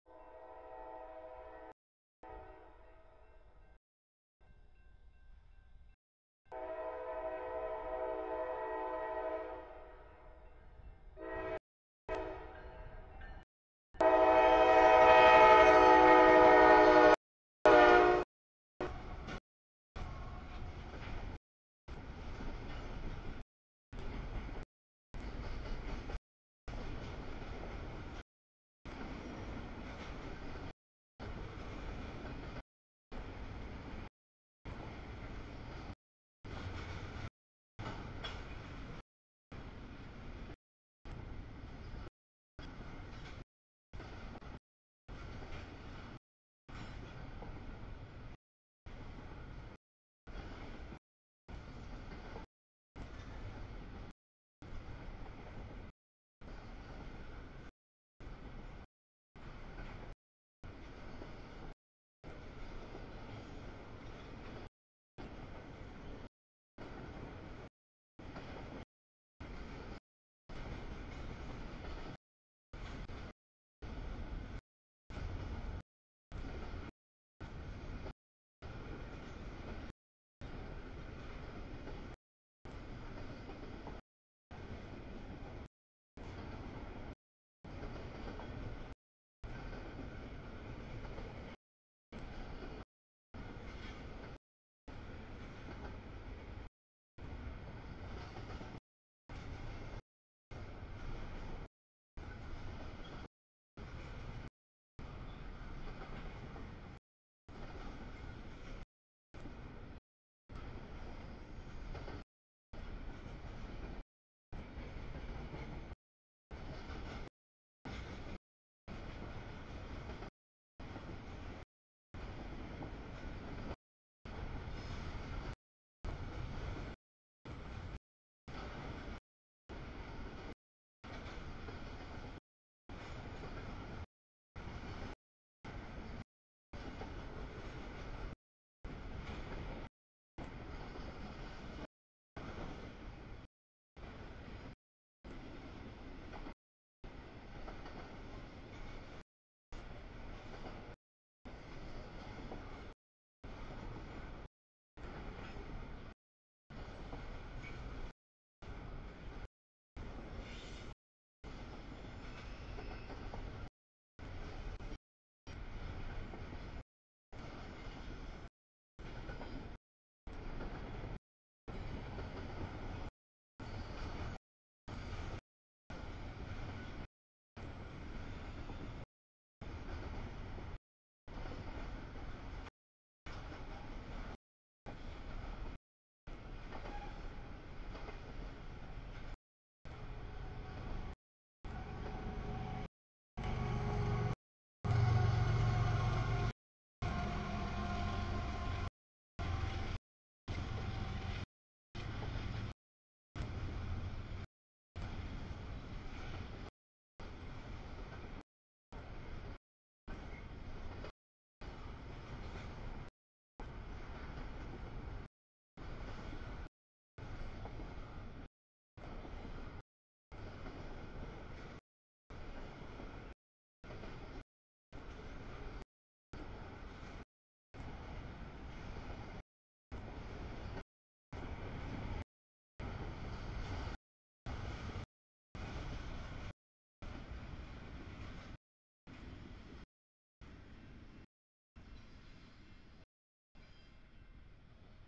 September 12 2019 Trains 6 PM to 12 AM
New Daily Format
6 PM to 12 AM trains
18:15 sb green trash, mix CSX 843 mDPU 731 Q439
19:39 sb mix CSX Q403
21:47 nb stack CSX L167
22:35 sb UPS trailers CSX Q002
23:33 sb stack CSX mDPU Q004